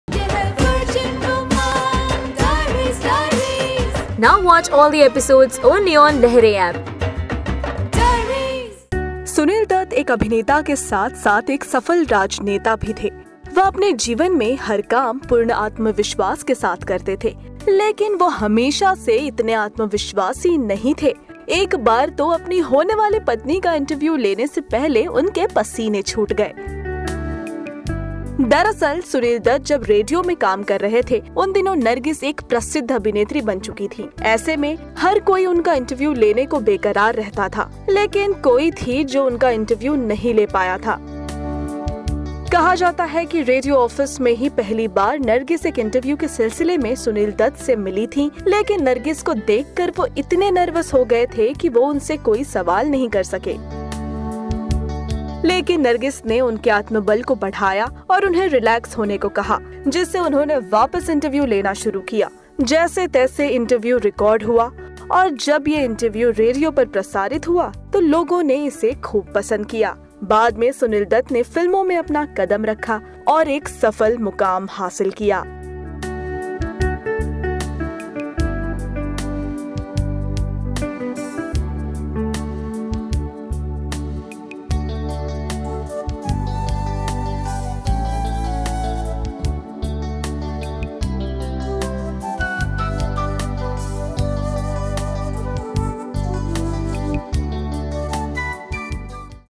When Sunil Dutt Got Nervous After Seeing Nargis In A Radio Interview
Sunil  Dutt began as a radio jockey interviewing film stars on Radio Ceylon before entering into movies. But, did you know that once he got so nervous seeing Nargis in a Radio interview that he cancelled the interview. About Lehren: